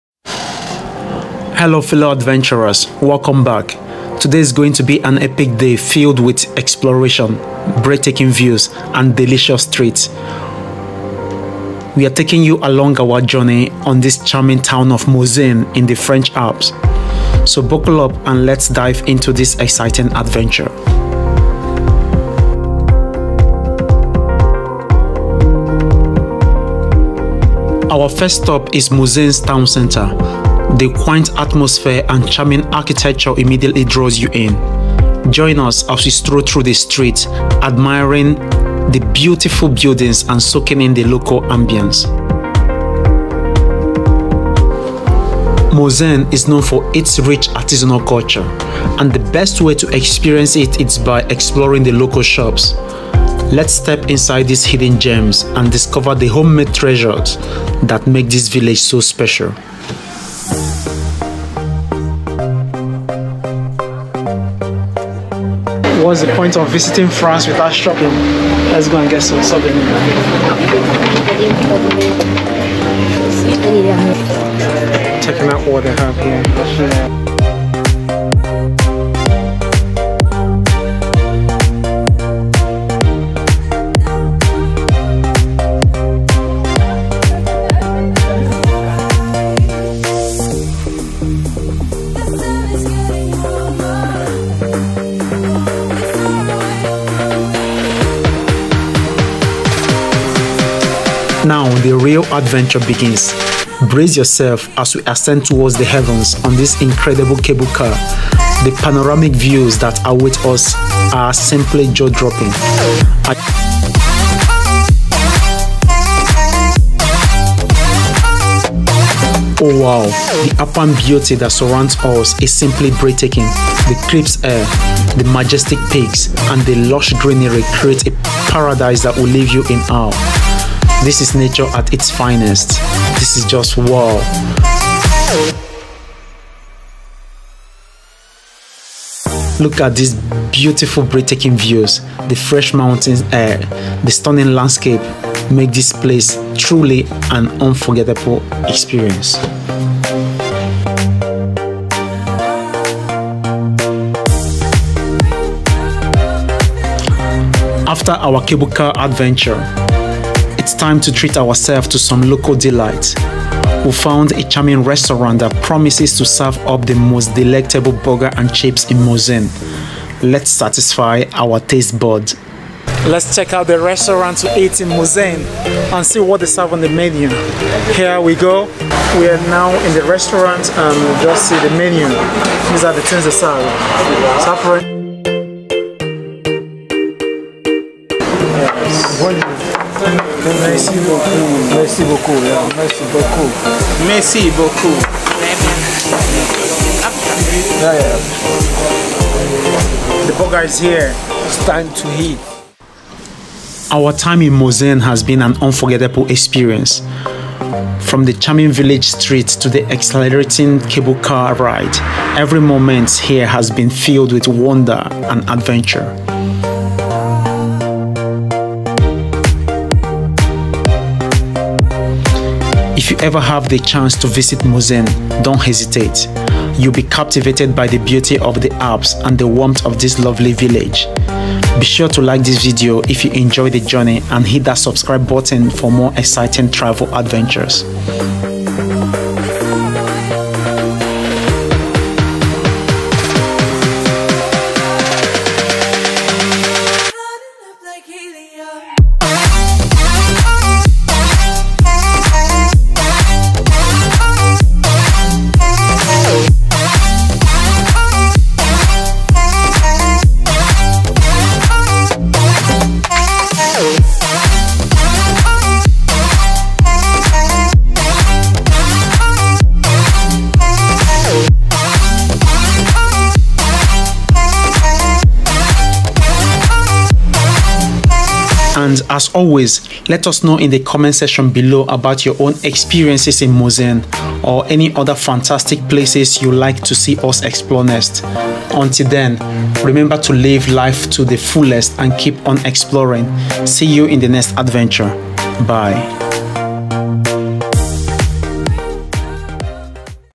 Exploring the Enchanting Village of Morzine | A French 🇫🇷 Alpine Adventure
Get ready for an unforgettable adventure in the enchanting village of Morzine nestled in the French Alps! Join us as we explore the picturesque streets adorned with charming chalets and colorful flowers. Discover the local treasures in quaint shops and soak in the warm atmosphere of this welcoming community.

But the real highlight awaits as we embark on an awe-inspiring cable car ride to the heavens. Feast your eyes on the breathtaking alpine beauty, with majestic peaks and lush greenery stretching as far as the eye can see.